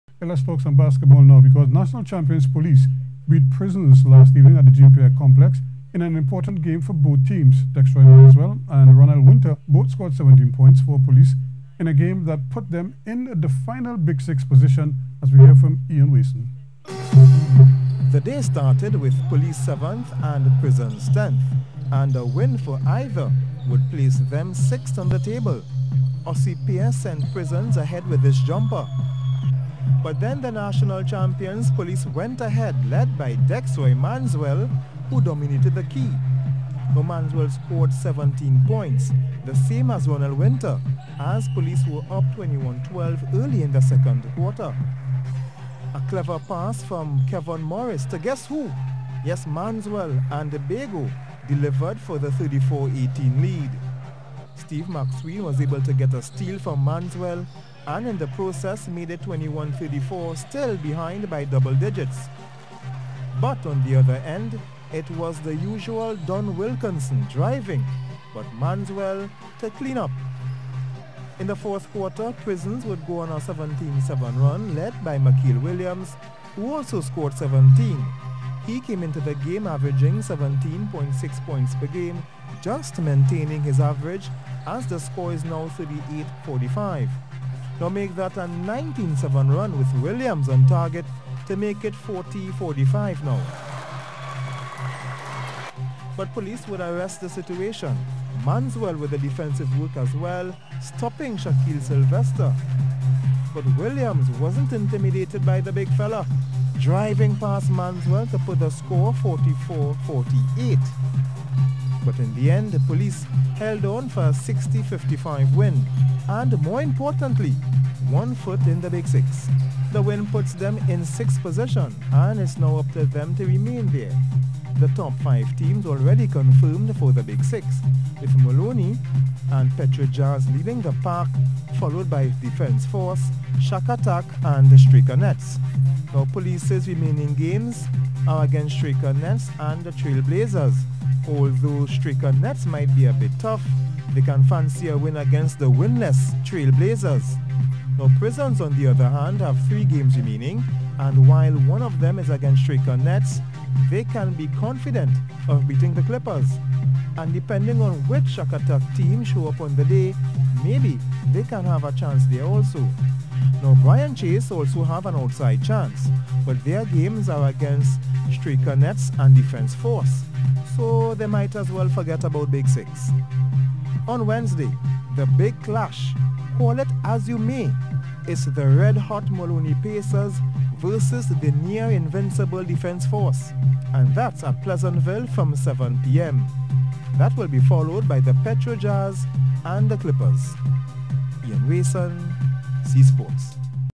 Police Closer To Big 6
National Basketball Champions, Police, beat Prisons last evening at the Jean Pierre Sports Complex in an important game for both teams.

Dextroy Manswell and Ronell Winter both scored 17 points for Police in a game that put them in the Final Big 6 position, as Ian Wason explains.